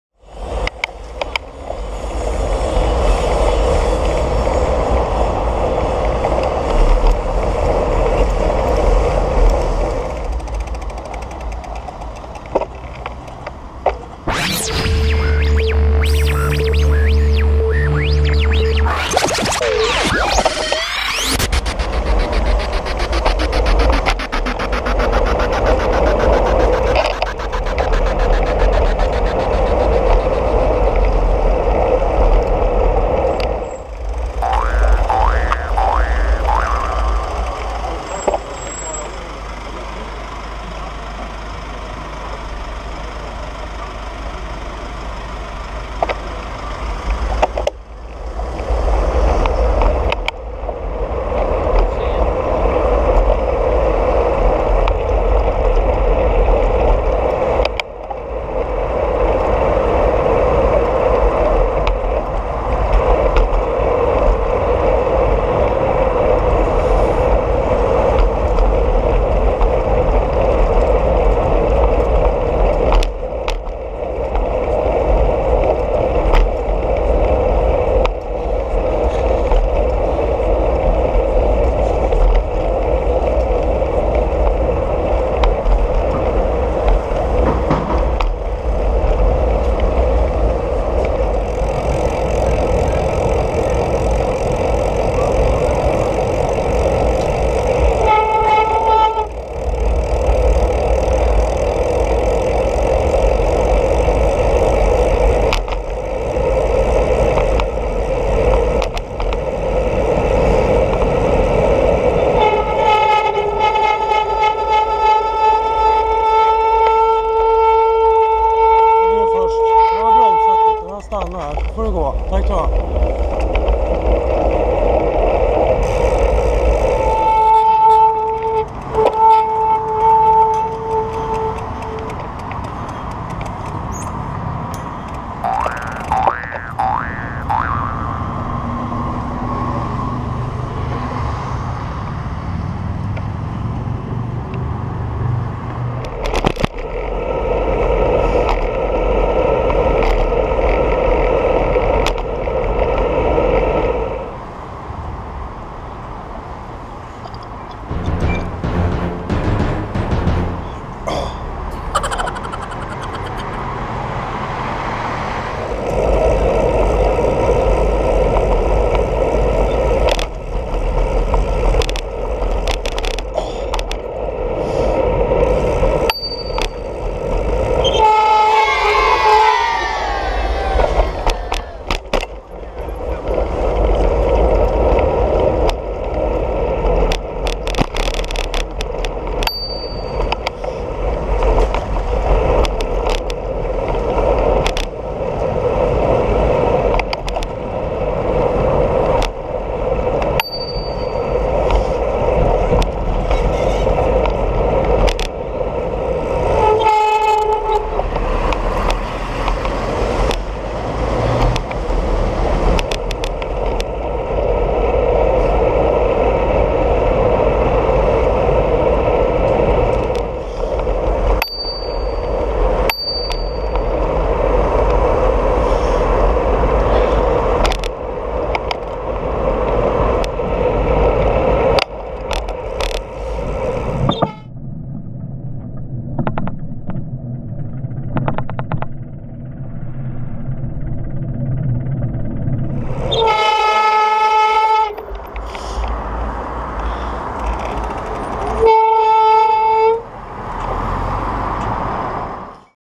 Electric bike!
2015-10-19 at lunchtime. Electric bikes are nice, running the red lights are not!